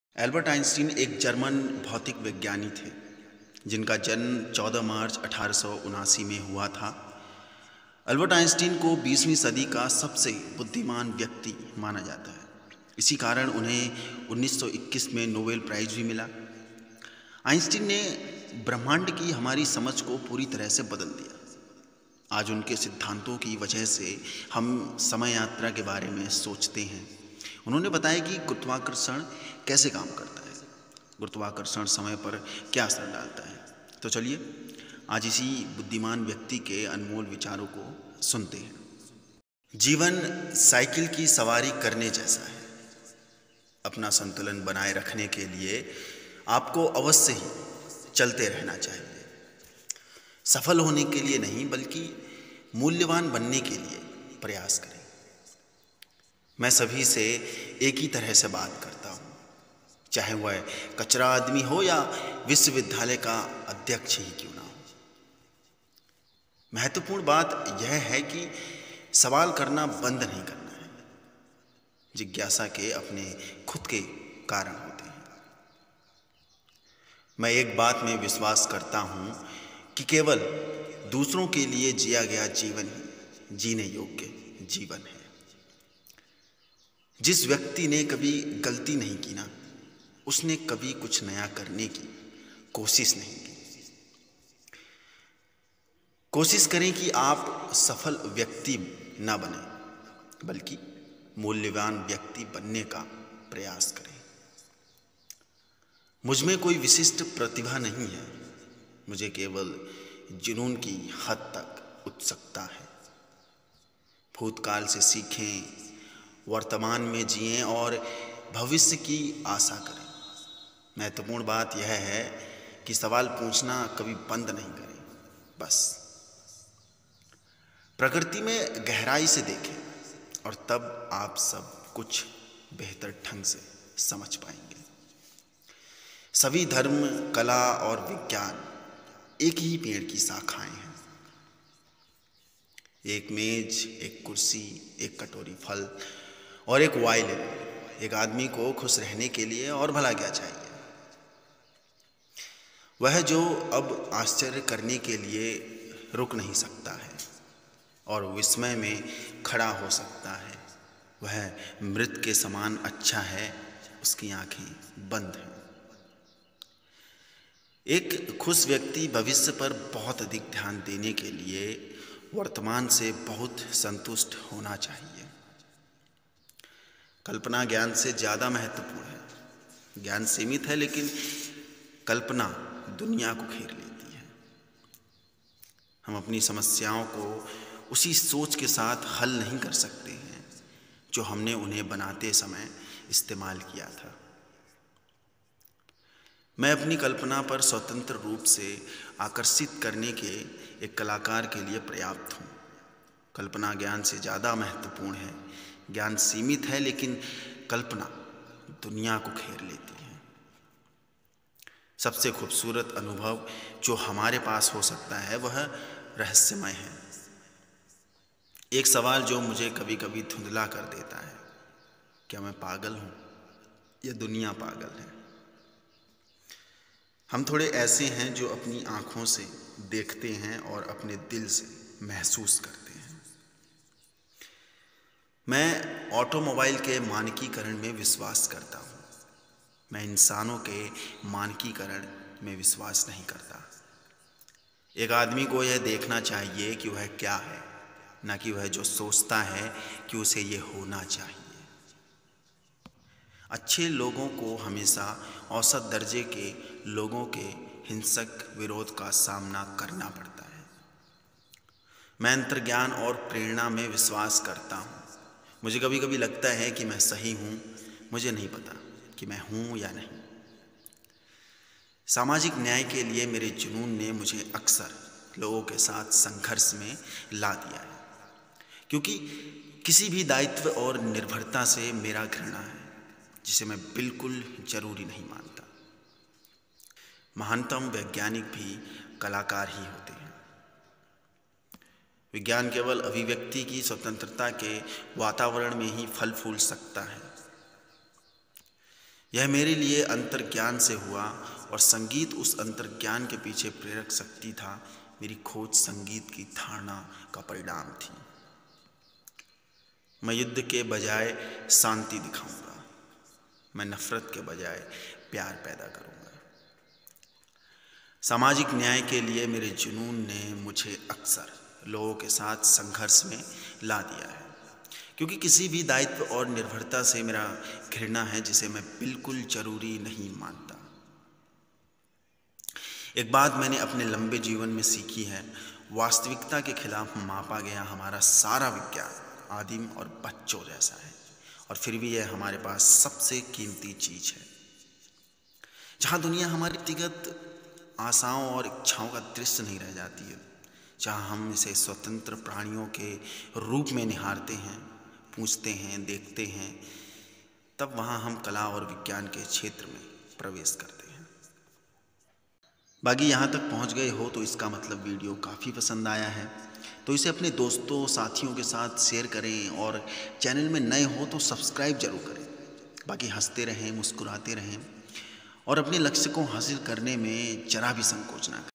अल्बर्ट आइंस्टीन के श्रेष्ठ अनमोल विचार-Best Albert Einstein Quotes in Hindi
अल्बर्ट आइंस्टीन एक जर्मन भौतिक विज्ञानी थे, जिनका जन्म 14 मार्च 1879 में हुआ था! अल्बर्ट आइंस्टीन को बीसवीं सदी का सबसे बुद्धिमान व्यक्ति माना जाता है। इसी कारण उन्हें 1921 में नोबेल प्राइज भी मिला।
आइंस्टीन ने ब्रह्मांड की हमारी समझ को पूरी तरह से बदल दिया। आज उनके सिद्धान्तों की वजह से हम समय यात्रा के बारे में सोचते है। उन्होंने बताया की गुरुत्वाकर्षण कैसे काम करता है, गुरुत्वाकर्षण समय पर क्या असर डालता है!
तो चलिए आज इसी बुद्धिमान व्यक्ति के अनमोल विचारों को सुनते हैं!

► Quotes Hindi Gyan में आपको दुनिया के महान लोगों के most popular बहुमूल्य विचार मिलेंगे।